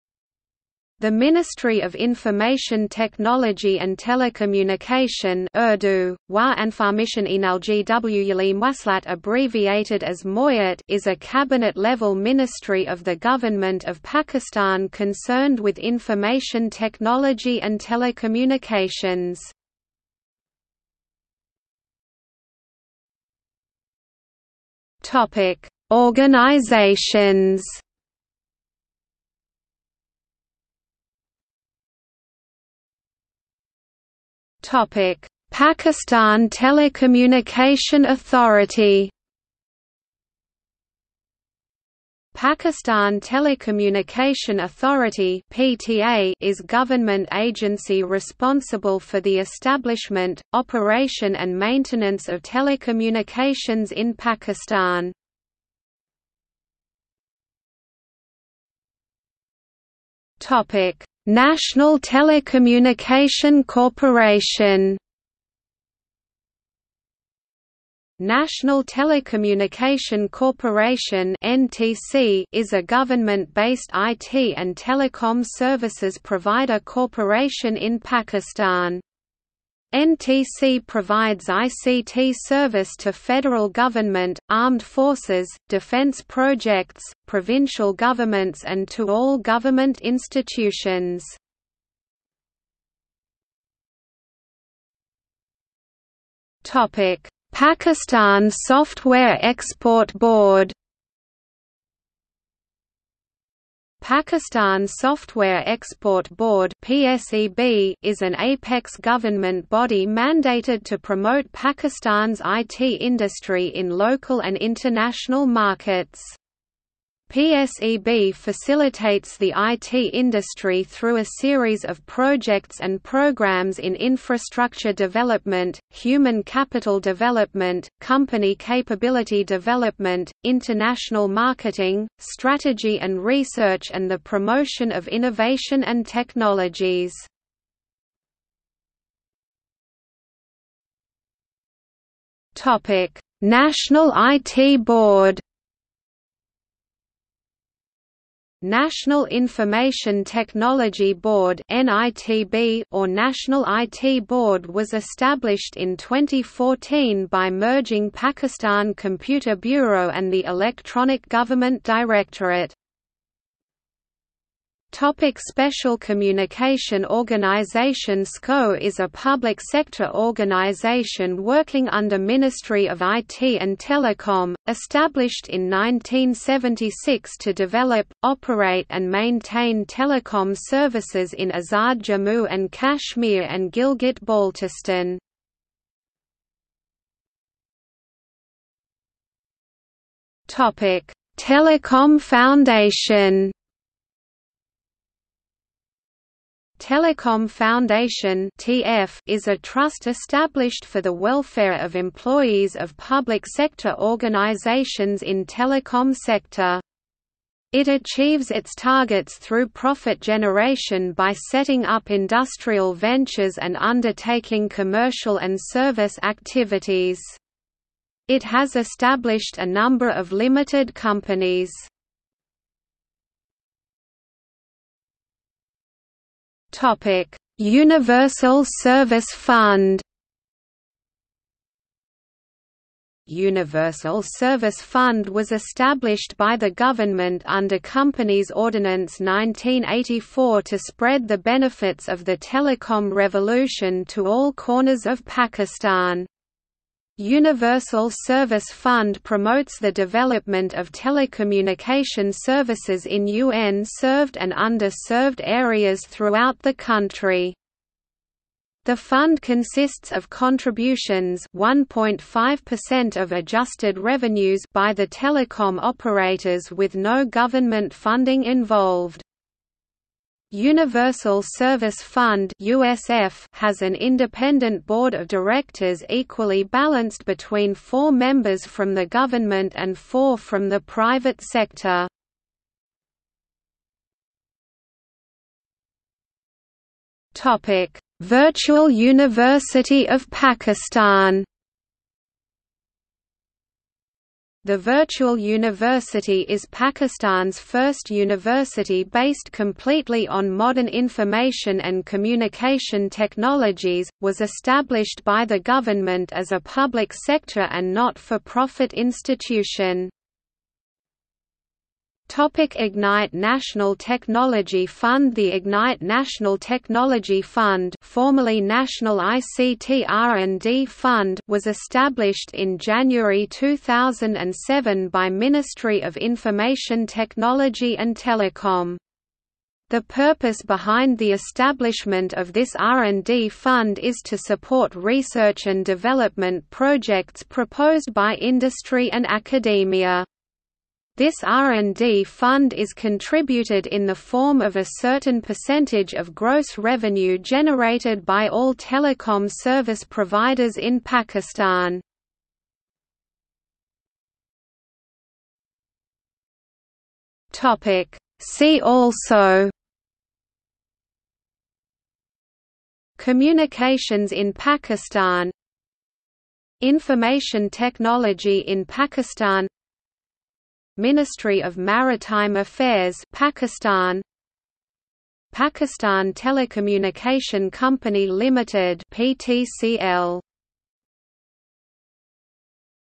Ministry of Information Technology and Telecommunication (Pakistan) | Wikipedia audio article
00:00:21 1 Organizations
00:00:31 1.1 Pakistan Telecommunication Authority
00:00:57 1.2 National Telecommunication Corporation
00:01:33 1.3 Pakistan Software Export Board
00:02:19 1.4 National IT Board
00:02:44 1.5 Special Communication Organization
00:03:07 1.6 Telecom Foundation
00:03:43 1.7 Universal Service Fund
00:04:44 1.8 Virtual University of Pakistan
00:05:12 1.9 Ignite National Technology Fund
00:05:54 2 See also

- Socrates

SUMMARY
The Ministry of Information Technology and Telecommunication (Urdu: وزارت انفارمیشن ٹیکنالوجی و ٹیلی مواصلات‬‎, abbreviated as MoITT) is a Cabinet-level ministry of the Government of Pakistan concerned with Information Technology and Telecommunications.